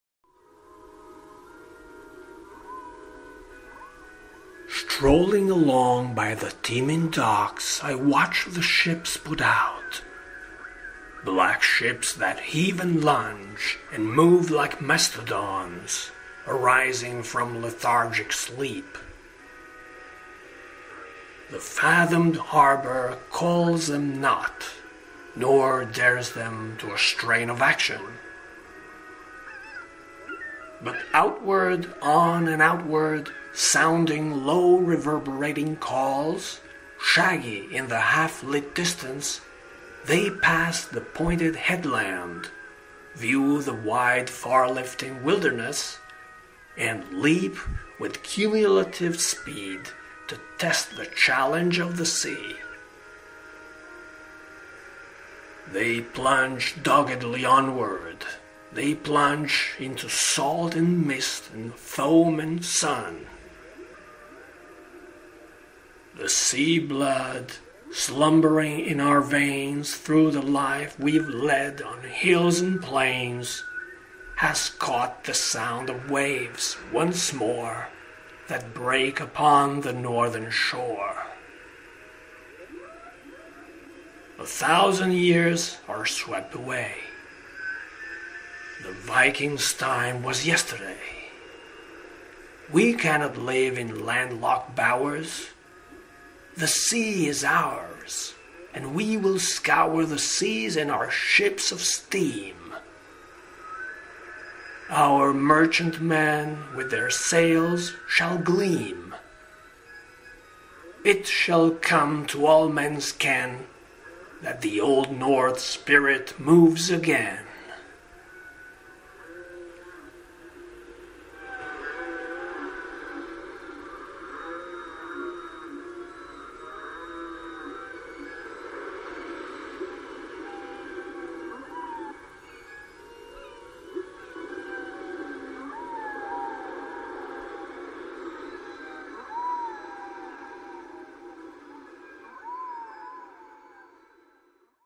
OUT TO SEA
This is a medley of Carl Sandburg's poem "Docks" and Oscar Williams's "The Spirit of the North". The video was filmed in Venice (Italy) in early September 2021.